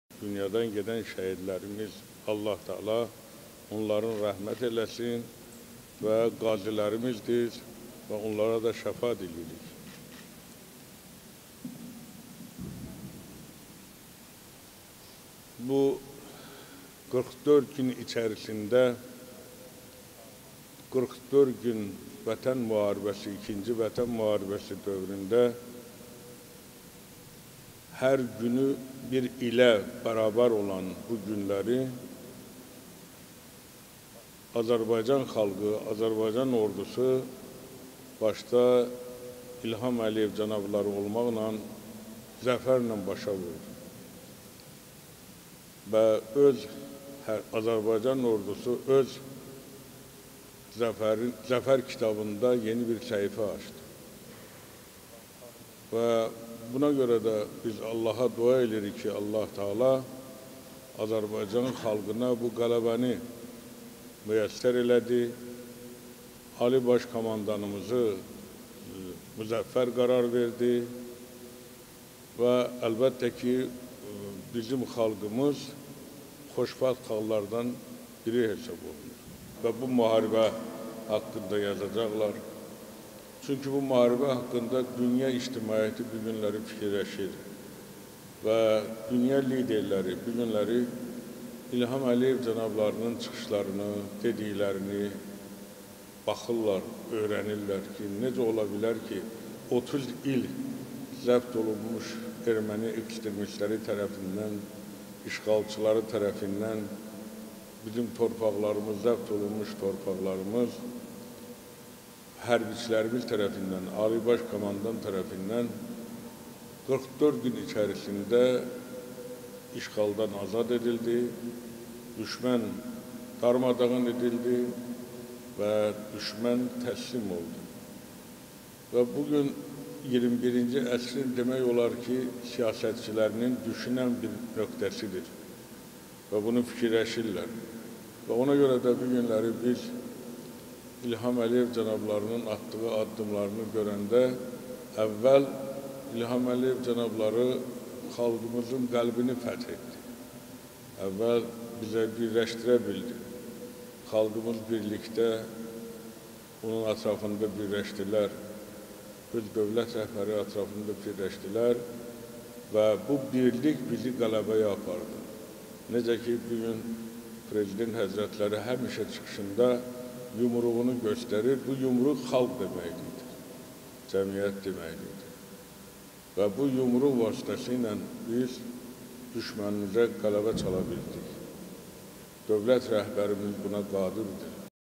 Allahşükür Paşazadə: Bu gün şəhidlərimizin ruhuna birlikdə dualar edirik
Allah, Vətən yolunda şəhid olanlar Ulu Tanrının yanında ən yüksək məqamdadırlar.AZƏRTAC xəbər verir ki, bunu Qafqaz Müsəlmanları İdarəsinin sədri Şeyxülislam Allahşükür Paşazadə Heydər məscidində Vətən müharibəsi şəhidlərinin xatirəsinə həsr olunmuş mərasimdən sonra bildirib.